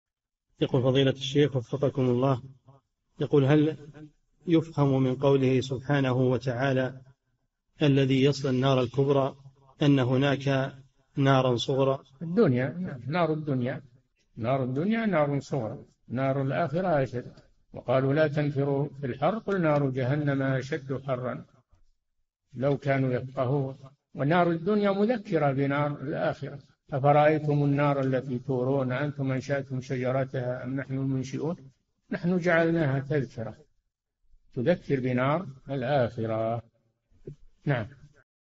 [3963- 4717] هل يفهم من قوله:( الذي يصلى النار الكبرى ) أن هناك نار صغرى؟ - الشيخ صالح الفوزان
"[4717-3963] هل يفهم من قوله سبحانه وتعالى: ﴿الَّذِي يَصْلَى النَّارَ الْكُبْرَى﴾ [الأعلى : 12] أنّ هناك نارًا صغرى؟
الجَوَابُ: في الدنيا، نار الدُّنيا، نار الدُّنيا نار صغرى، نار الآخرة أشدّ ﴿وَقَالُوا لَا تَنْفِرُوا فِي الْحَرِّ قُلْ نَارُ جَهَنَّمَ أَشَدُّ حَرًّا لَوْ كَانُوا يَفْقَهُونَ﴾ [التوبة : 81] ونار الدنيا مذكرة بنار الآخرة، ﴿أَفَرَأَيْتُمُ النَّارَ الَّتِي تُورُونَ (71) أَأَنْتُمْ أَنْشَأْتُمْ شَجَرَتَهَا أَمْ نَحْنُ الْمُنْشِئُونَ (72) نَحْنُ جَعَلْنَاهَا تَذْكِرَةً﴾ [الواقعة: 71 - 73] تُذكِّر بنار الآخرة.

للاشتراك في قناة الشيخ صالح الفوزان
فتاوى المجالس في تفسير المفصل

للاستفسار والإقتراحات والدعم
رئيس الفريق
لمشروع كبار العلماء